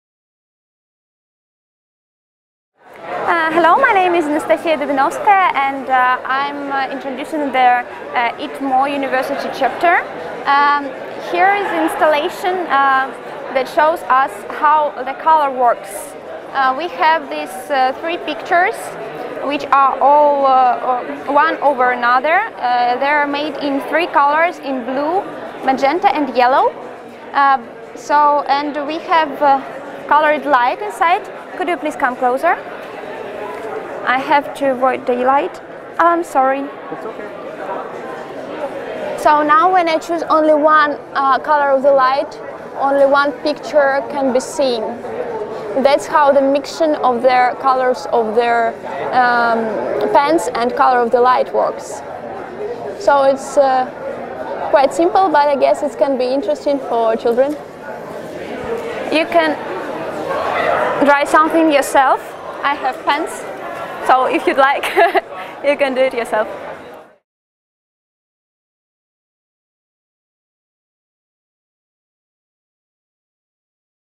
ITMO Univ. Chapter - Optical Painting
From the 2016 SPIE Optics Outreach Games held annually at SPIE Optics + Photonics Symposium.

Optical Painting by Anastasiia Dubinovskaia, Alaudi Denisultanov, Tatiana Vovk, and Solveyga Azbite from the ITMO University Student Chapter.